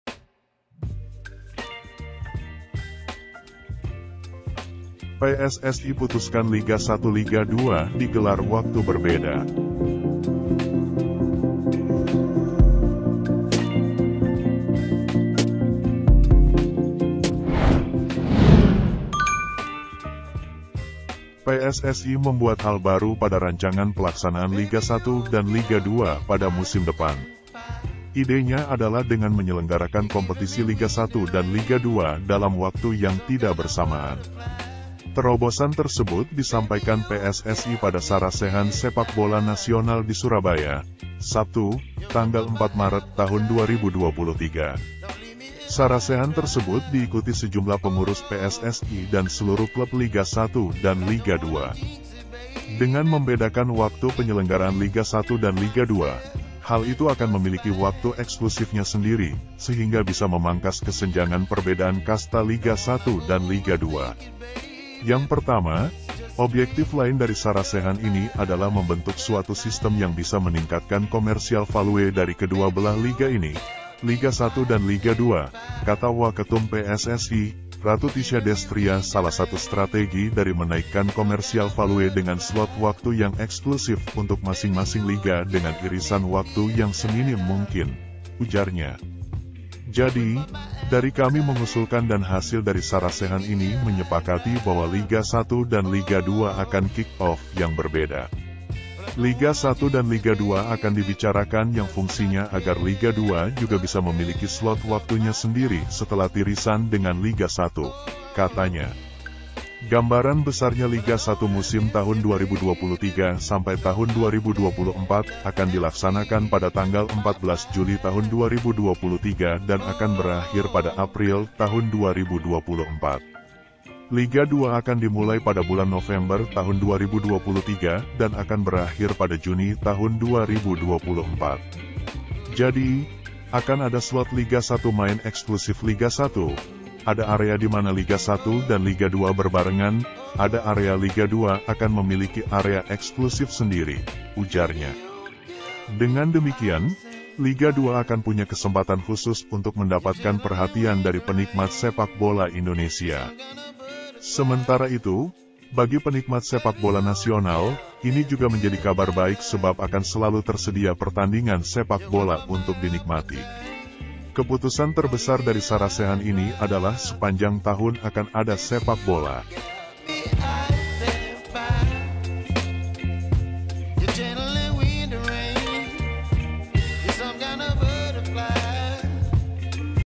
BREAKING NEWS SEPAKBOLA INDONESIA SEPANJANG TAHUN SARASEHAN SEPAKBOLA
Sarasehan Sepak Bola Nasional: PSSI Putuskan Liga 1-Liga 2 Digelar Waktu Berbeda. PSSI membuat hal baru pada rancangan pelaksanaan Liga 1 dan Liga 2 pada musim depan. Idenya adalah dengan menyelenggarakan kompetisi Liga 1 dan Liga 2 dalam waktu yang tidak bersamaan. Terobosan tersebut disampaikan PSSI pada Sarasehan Sepak Bola Nasional di Surabaya, Sabtu (4/3/2023). Sarasehan tersebut diikuti sejumlah pengurus PSSI dan seluruh klub Liga 1 dan Liga 2. Dengan membedakan waktu penyelenggaraan Liga 1 dan Liga 2, hal itu akan memiliki waktu eksklusifnya sendiri sehingga bisa memangkas kesenjangan perbedaan kasta Liga 1 dan Liga 2.

"Yang pertama, obyektif lain dari sarasehan ini adalah membentuk suatu sistem yang bisa meningkatkan komersial value dari kedua belah liga ini, Liga 1 dan Liga 2," kata Waketum PSSI, Ratu Tisha Destria "Salah satu strategi dari menaikkan komersial value dengan slot waktu yang eksklusif untuk masing-masing liga dengan irisan waktu yang seminim mungkin," ujarnya. "Jadi, dari kami mengusulkan dan hasil dari sarasehan ini menyepakati bahwa Liga 1 dan Liga 2 akan kick-off yang (berbeda). Liga 1 dan Liga 2 akan dibicarakan yang fungsinya agar Liga 2 juga bisa memiliki slot waktunya sendiri setelah tirisan dengan Liga 1," katanya. Gambaran besarnya Liga 1 musim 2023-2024 akan dilaksanakan pada 14 Juli 2023 dan akan berakhir pada April 2024. Liga 2 akan dimulai pada bulan November 2023 dan akan berakhir pada Juni 2024.

"Jadi, akan ada slot Liga 1 main eksklusif Liga 1, ada area di mana Liga 1 dan Liga 2 berbarengan, ada area Liga 2 akan memiliki area eksklusif sendiri," ujarnya. Dengan demikian, Liga 2 akan punya kesempatan khusus untuk mendapatkan perhatian dari penikmat sepak bola Indonesia. Sementara itu, bagi penikmat sepak bola nasional, ini juga menjadi kabar baik sebab akan selalu tersedia pertandingan sepak bola untuk dinikmati. "Keputusan terbesar dari sarasehan ini adalah sepanjang tahun akan ada sepak bola. Sepanjang tahun akan menikmati sepak bola, mulai Liga 1 atau Liga 2," tutur Ratu Tisha. "Kita harapkan dengan hal ini kualitas kompetisi akan naik, kualitas komersial juga akan meningkat, dan ujungnya pada pembinaan timnas dan kesejahteraan klub Liga 1 dan Liga 2," katanya.